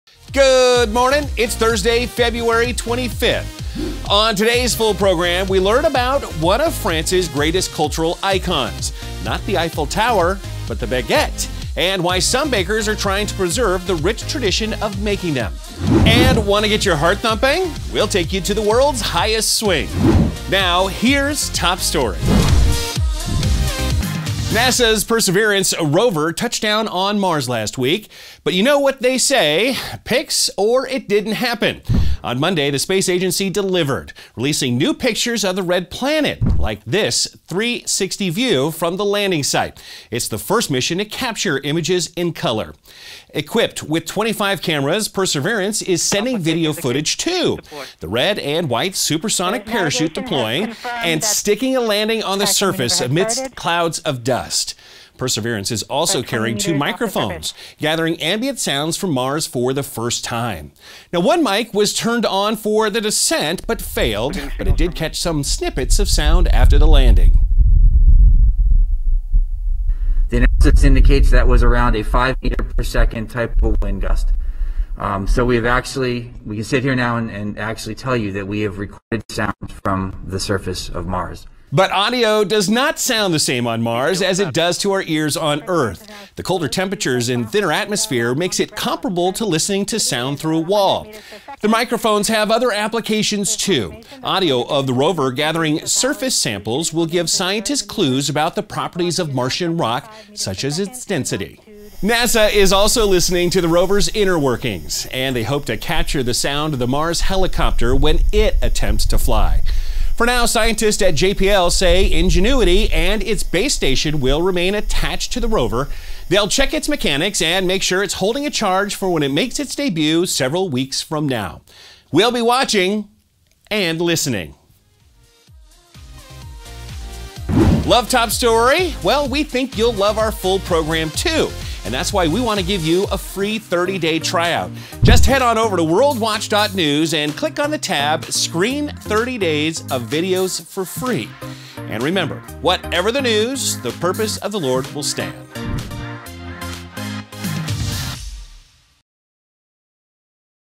Red rover, red rover, send footage on over! | Top Story
NASA releases never-before-seen (or heard) footage from Mars.